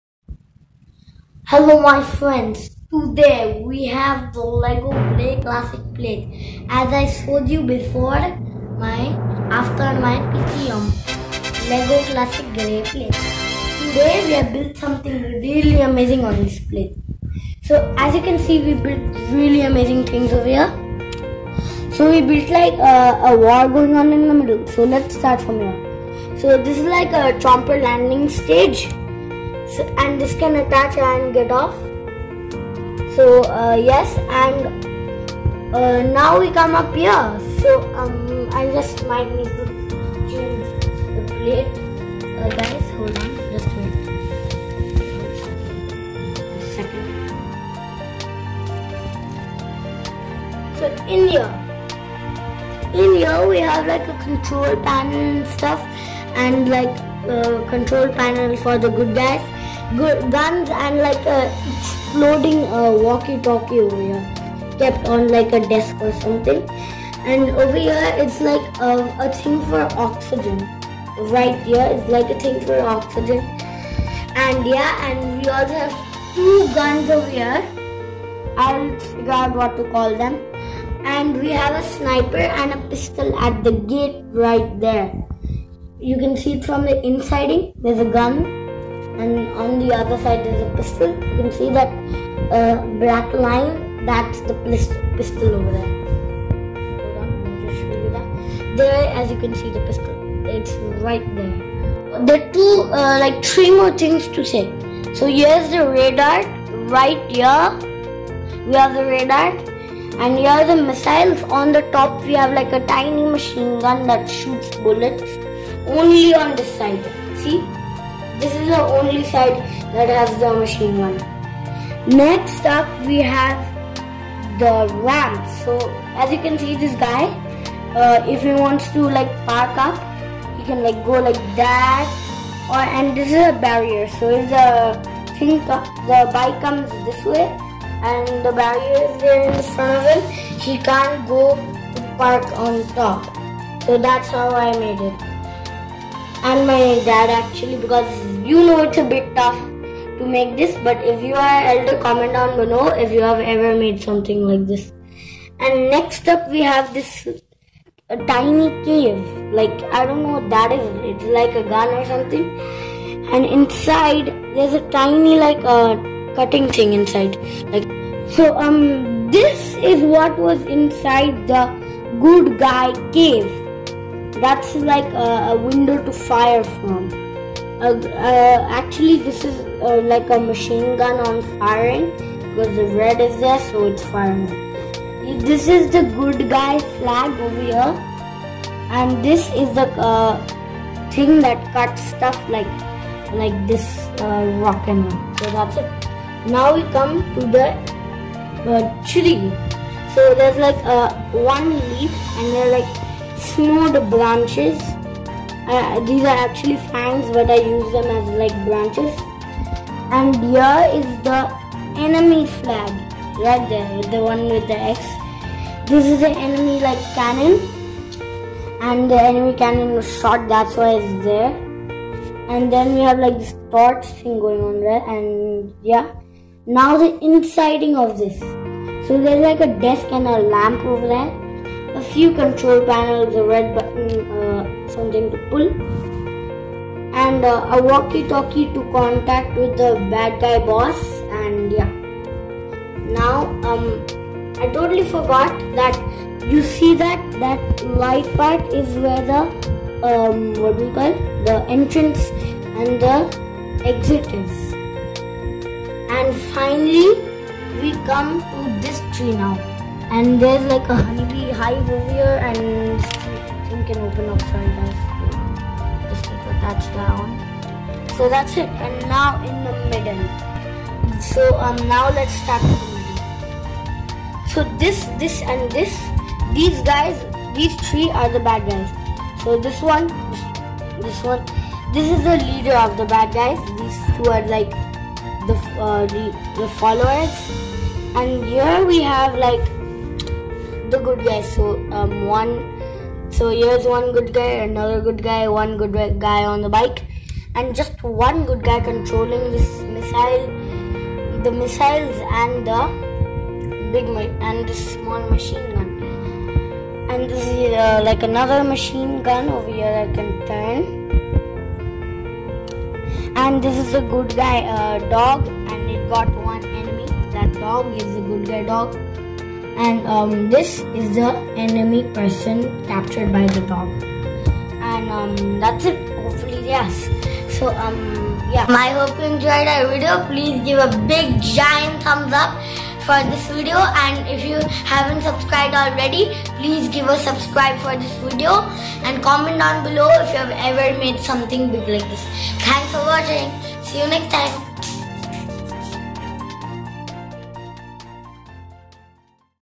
Lego Army Base : Playable Creation on Lego Baseplate
This army base was built on a 48x48 stud Lego baseplate. It includes a helicopter landing pad. Headquarters with a radar and missiles. Inside the headquarters is a control desk and mounting point for various accessories like guns and oxygen cylinders. A ramp leads up to the roof to park a motorcycle. The snowy mountain has an entrance and machine gun mounted on the roof. The hideout of the bad guys is inside a hill and has a table for planning. The roof features a cannon.